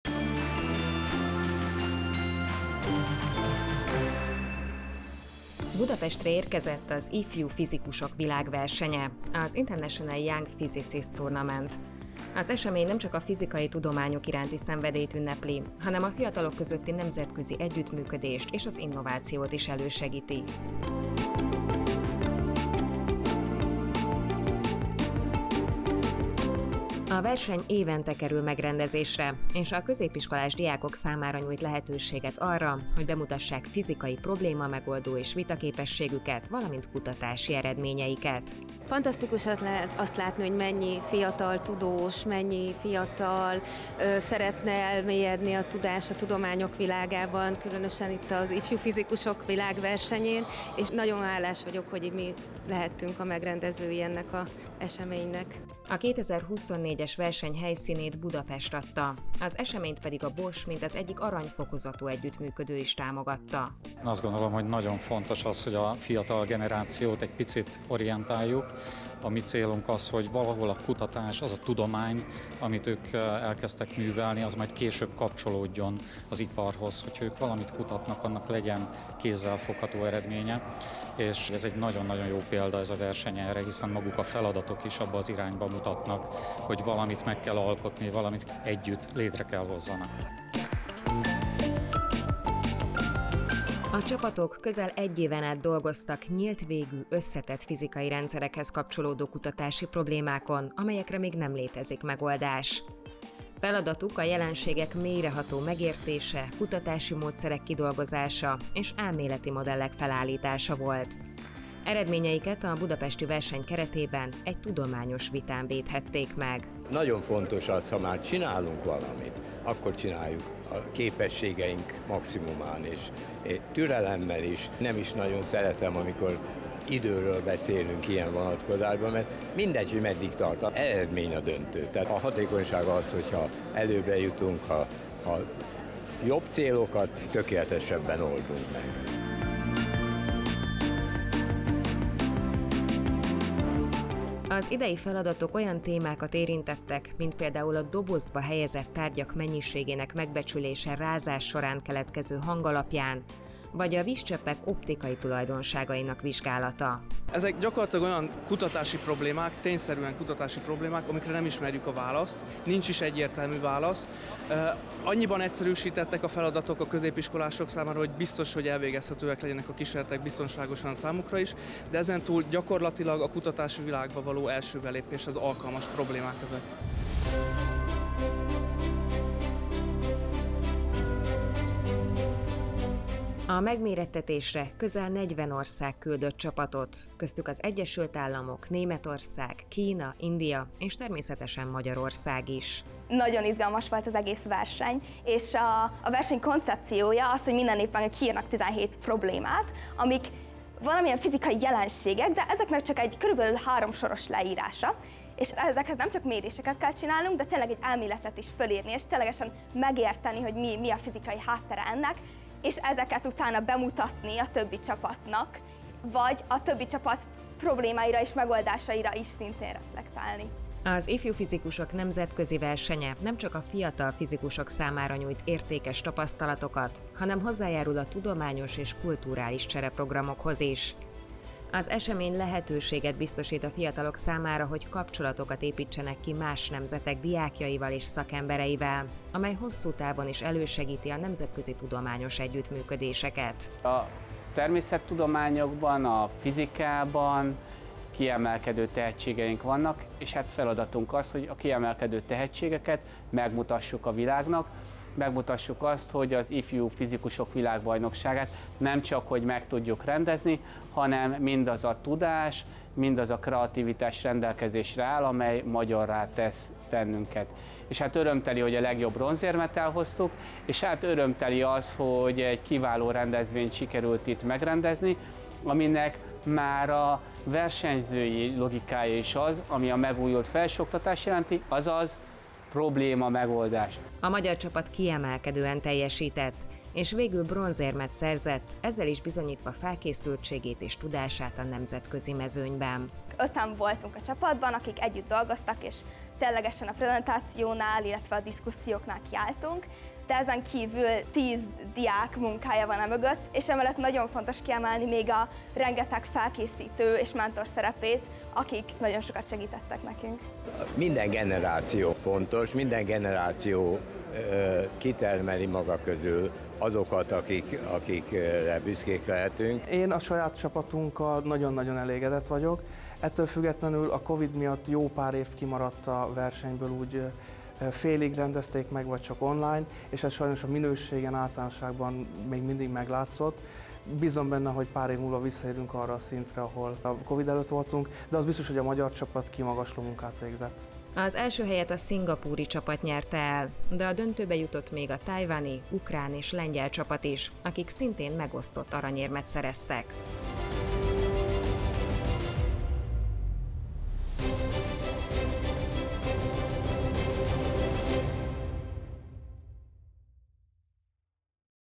Ifjú Fizikusok Nemzetközi Versenye (IYPT 2024)
Az idei nyár egyik jelentős eseménye volt a Budapesten megrendezett Ifjú Fizikusok Nemzetközi Versenye, ami nem egy klasszikus értelemben vett fizikaverseny. A szervezők minden évben 17 nyílt végű, középiskolások által is megérthető kutatási problémát tűznek ki, amelyekben alaposan elmélyedhetnek az érdeklődő diákok, majd pedig a kommunikációs és vitakultúrájukat is fejleszthetik. A versenyt Szingapúr csapata nyerte, a hazai csapat bronzérmes lett.